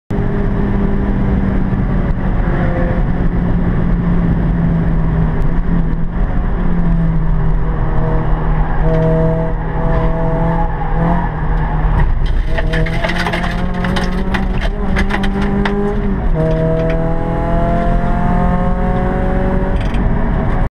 Donington Park 18th July 2015 - Small Off
When you lapse in concentration but still keep your foot in.